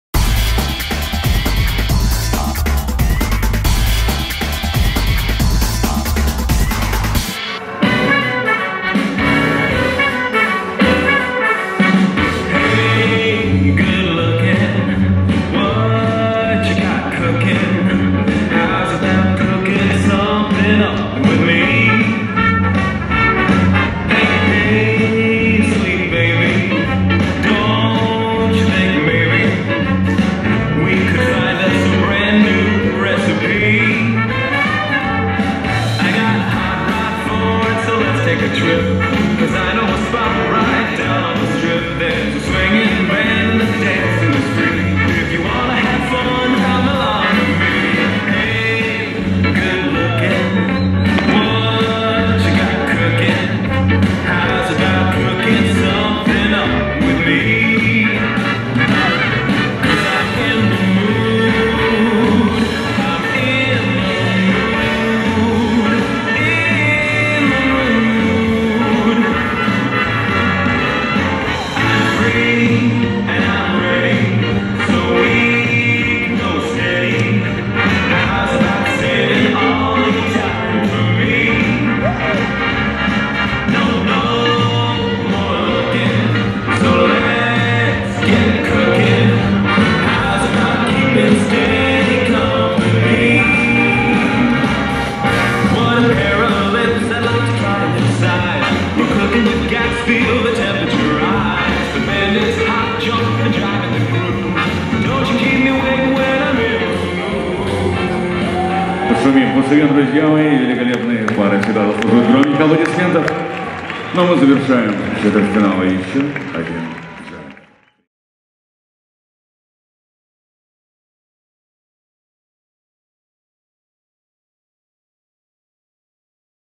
Jive | Evgeny Nikolaev - Ekaterina Isakova | Russian Championship Amateur Latin 2021
Russian Championship Amateur Latin 2021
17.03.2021 Moscow, Gymnstic Palace Irina Viner-Usmanova

1. Armen Tsaturyan - Svetlana Gudyno
2. Andrey Gusev - Vera Bondareva
3. Anton Aldaev - Natalia Polukhina
4. Semyon Khrzhanovsky - Elizaveta Lykhina
5. Maxim Elfimov - Evgenia Churikova
6. Egor Petrov - Daria Petrova